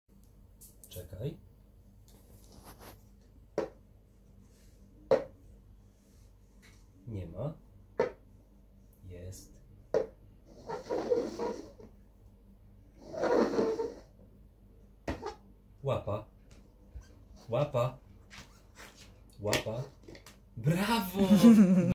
Cocker Spaniel cups game / gra w kubeczki
Our Spaniel loves this game. what about yours:)
We practiced for a few minutes only. This breed is very intelligent.

Nasza kochana Sunia uwielbia gre w kubeczki, a Wasze?
Uczylismy sie kilka minut, to  bardzo inteligentna rasa.